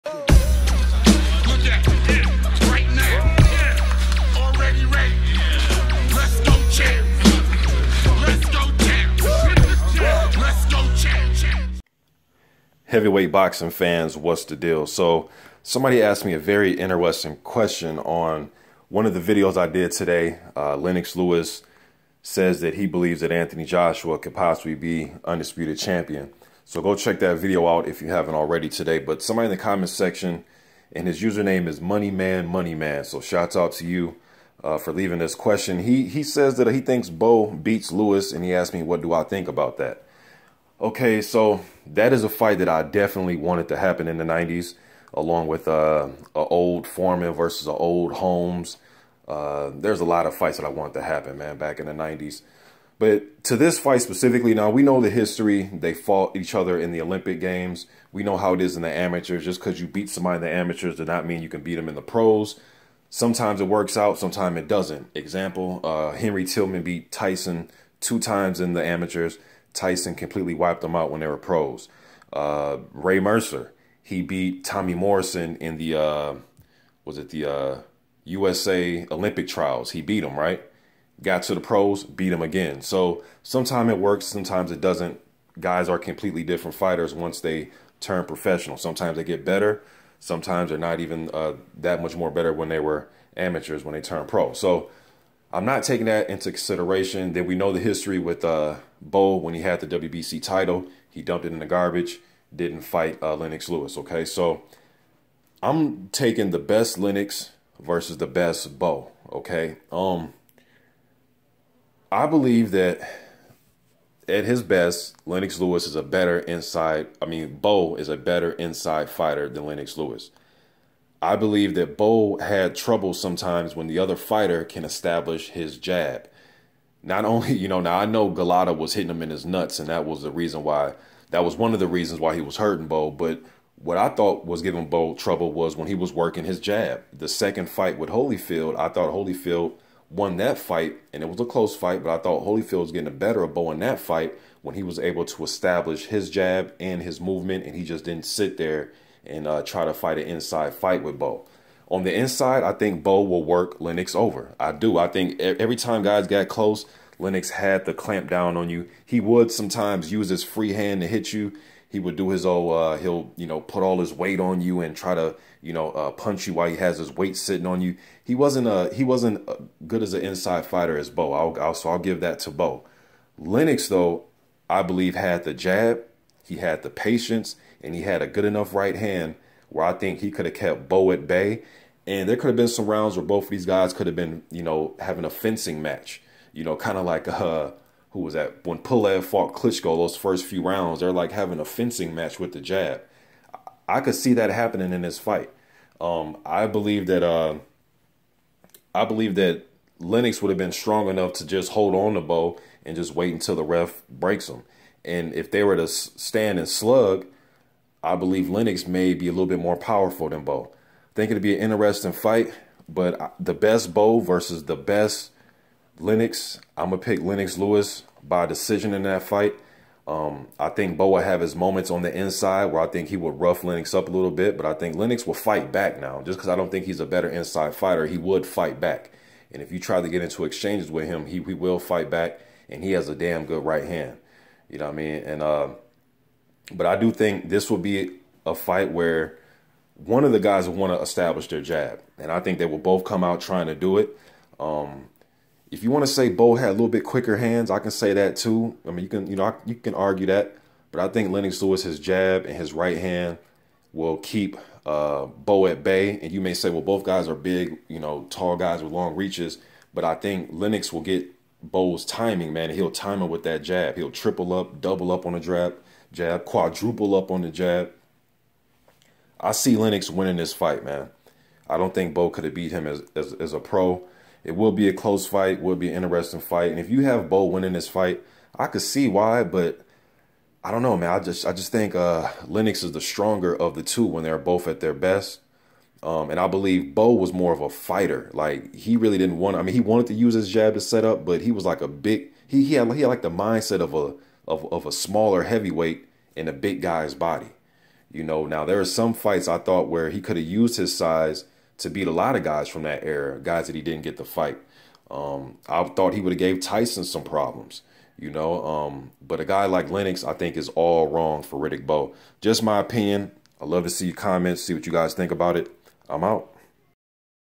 Riddick "Big Daddy" Bowe vs Lennox "The Lion" Lewis - In Their Prime Who Wins?
MrBOXINGTODAY2 SPEAKS ON A MYTHICAL MATCH-UP BETWEEN TWO FORMER UNDISPUTED HEAVYWEIGHT CHAMPIONS RIDDICK BOWE VS LENNOX LEWIS IN THEIR PRIMES!